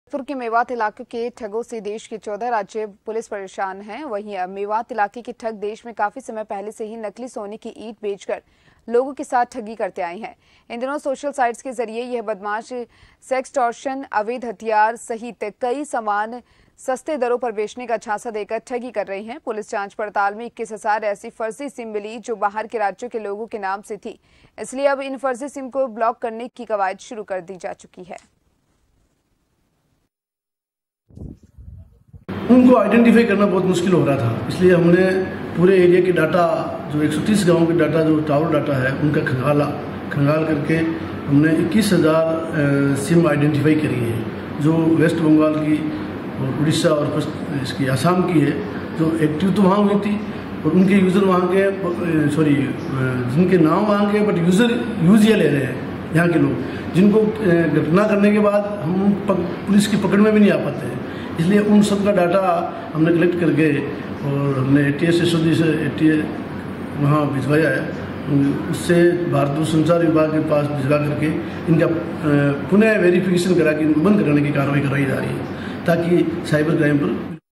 Bharatpur मे मेवात ठगो की फर्जी मोबाइल सिम को Police ने किया चिन्हित, दूरसंचार और एटीएस को लिखा पत्र
News Credit-GS